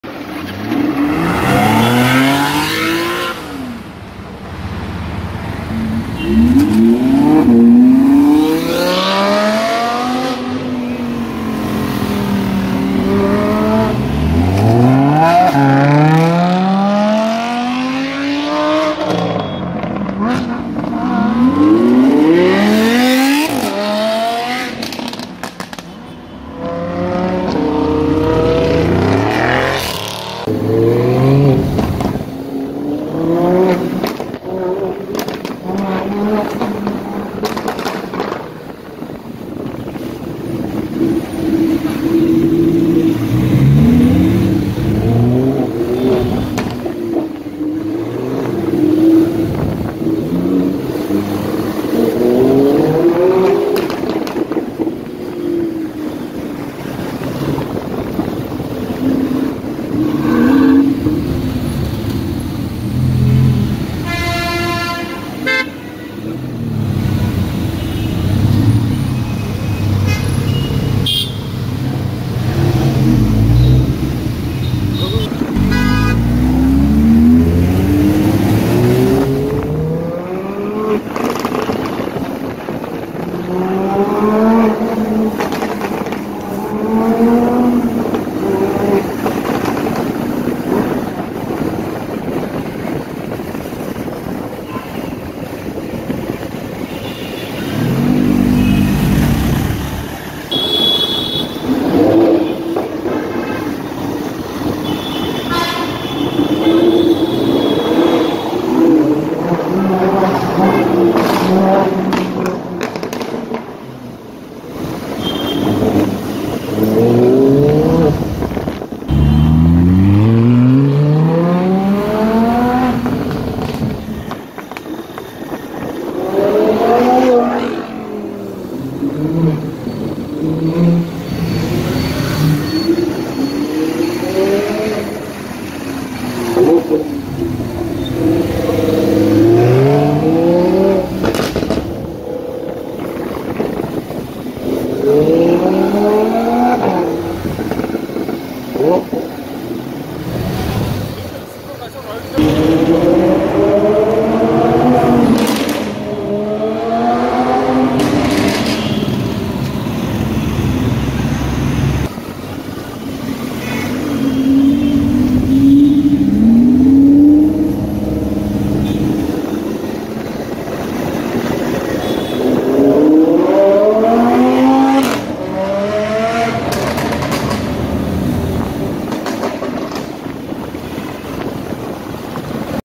3 Lamborghini's Gone Crazy In Hyderabad| India
3 Lamborghini Huracan's In Hyderabad
Terrorizing The Streets